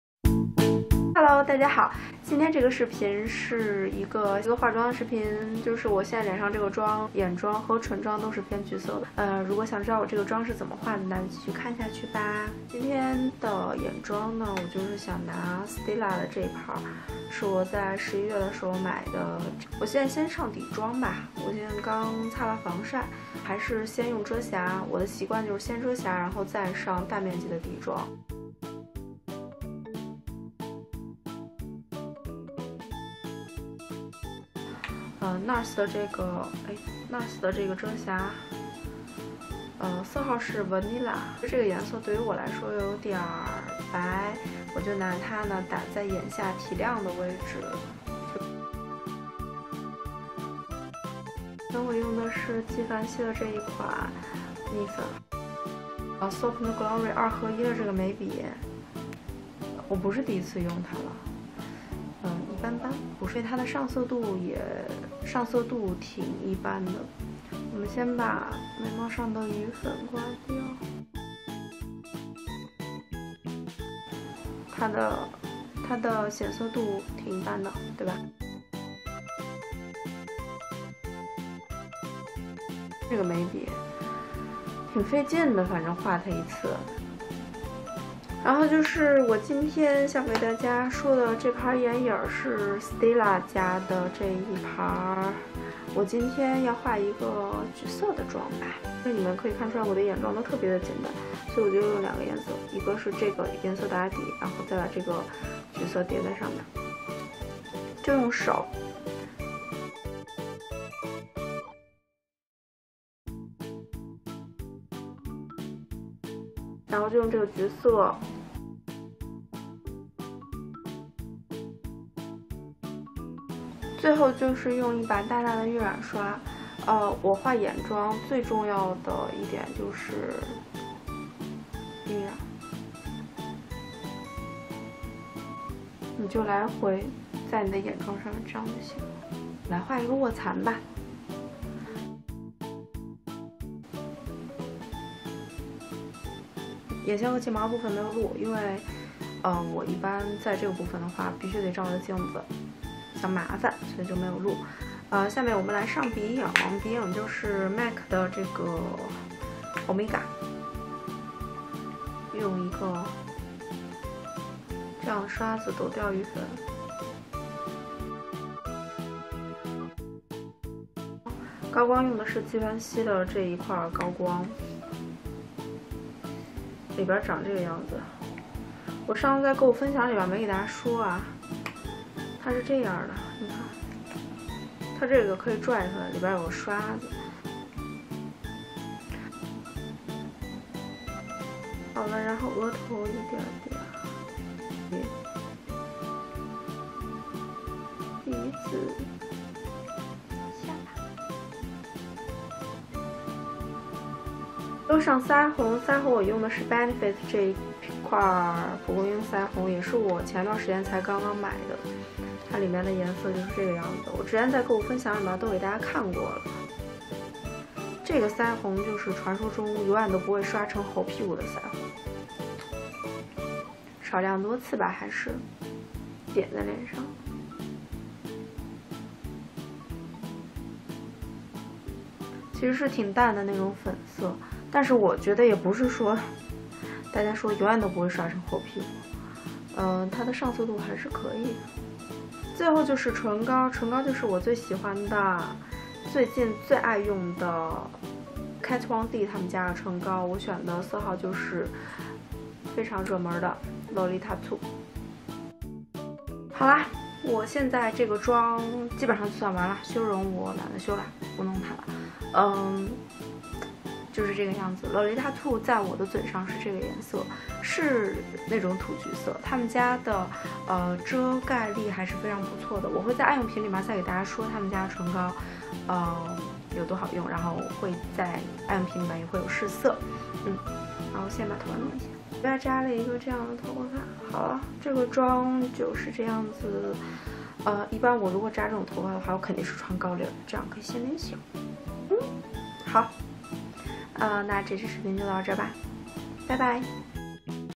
十一月战利品妆容分享
❤weibo,Youtube,Youku：Camille卡妹

这支视频是很久之前拍的，现在才剪出来，用到的产品基本上是我去年11月买到的彩妆品，希望你们能喜欢

产品详情在这里：

1. 遮瑕：collection lasting perfection concealer 色号：3

NARS radiant creamy concealer 色号：vanilla

2. 散粉：纪梵希四宫格散粉

3. 眉笔：soap&glary archery 2in1 眉笔 色号：hot chocolate

4. 眼影：stila eyes are the window palette 色号：spirit

5. 鼻影：mac单色眼影 omega

6. 高光：givenchy le prisme visage 纪梵希高光

7. 腮红：benefit dandelion blusher 贝玲妃蒲公英腮红

8. 唇彩：kat von d 液体唇彩 色号：lolita2

本视频所有产品均为自购 This video is NOT sponsored.